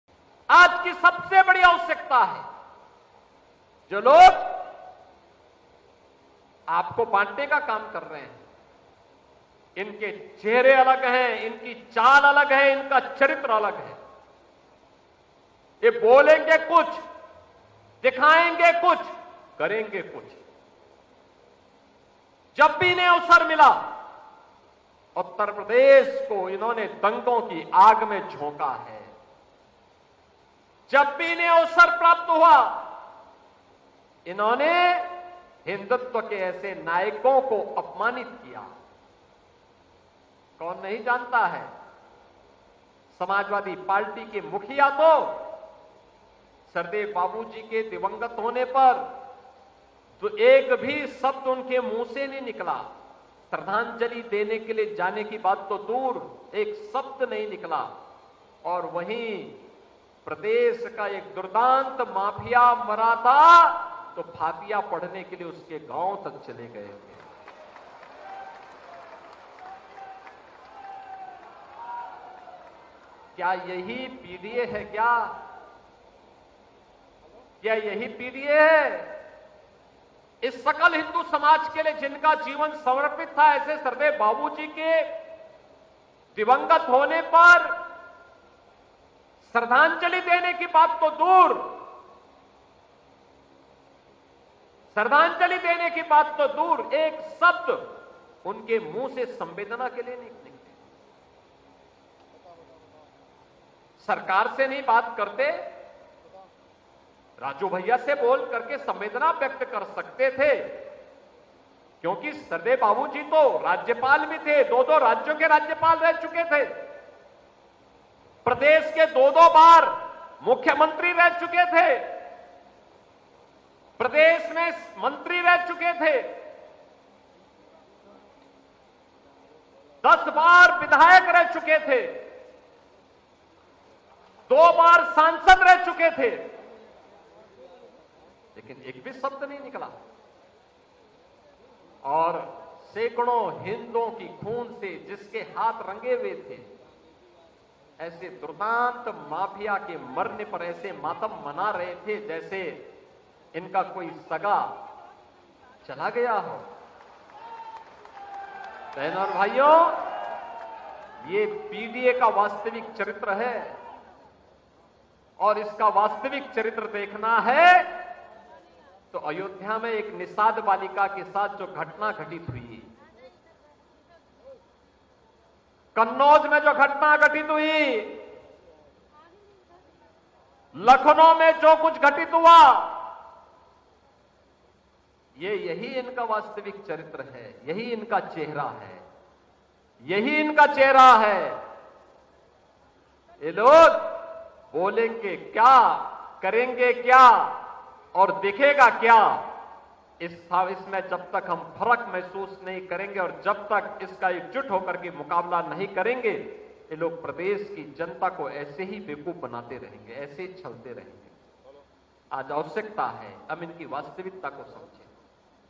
CM Yogi On Samajwadi Party : 'माफिया के मरने पर मातम मना रहे थे...' | CM Yogi Vs Akhilesh | Viral
Airtel - 350,
Netvision - 219,
RCN - 403,
UCN-223
Alisha Digital- 285
HDS- 127

We are also available on:
MX Player
JIO TV